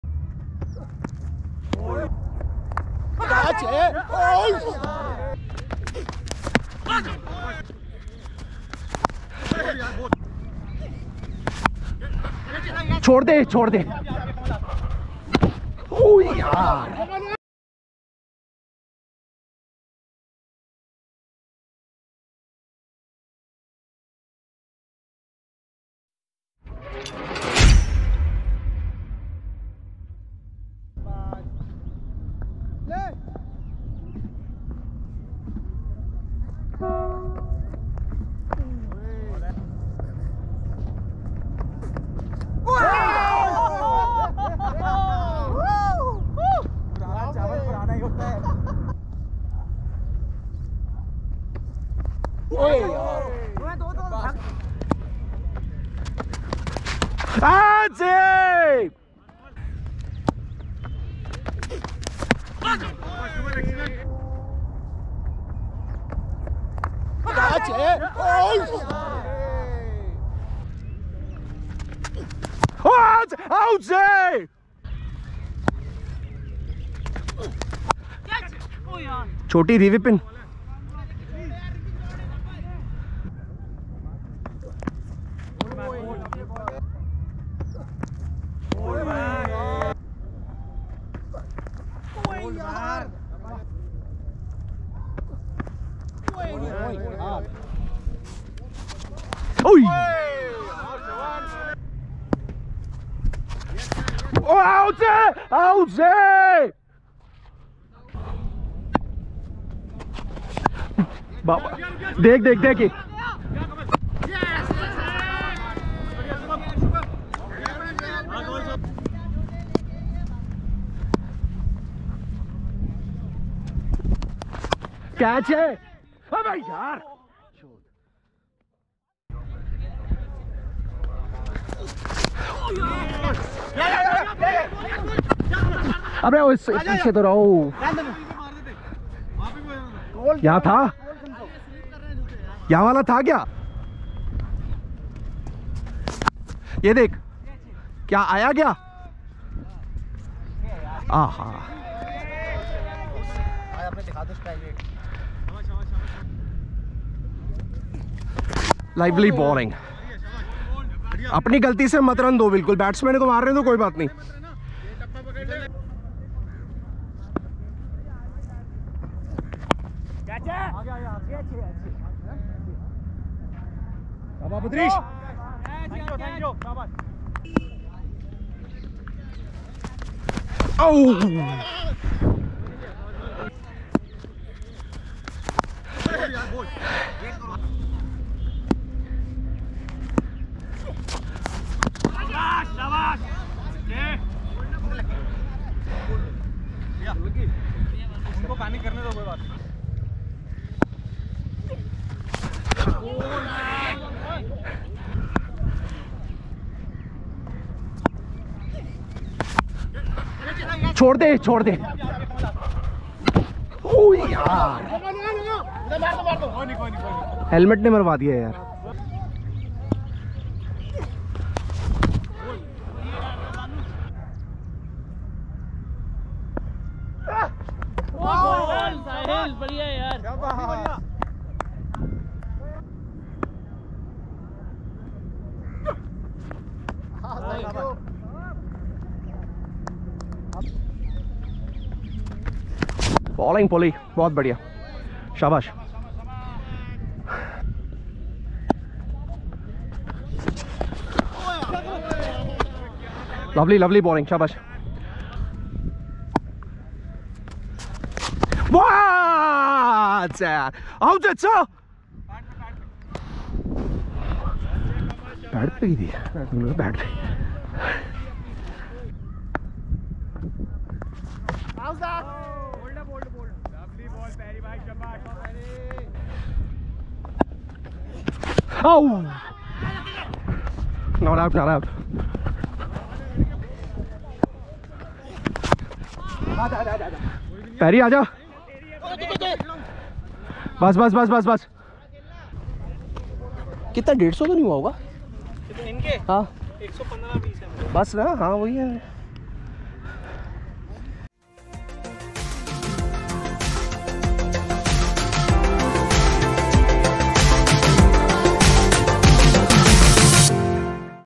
Hero GoPro Wicket Keeper Helmet Camera Cricket Highlights | HUGE Swing & SMOOTH Spin
This is Full GoPro POV From Wicket-keeper - Kamal Aswani. See what its like to face great bowling attack from Kamal's POV

GoPro Cricket Match

GoPro POV Cricket | Helmet Cam |

AXAXL vs MV XI Bulls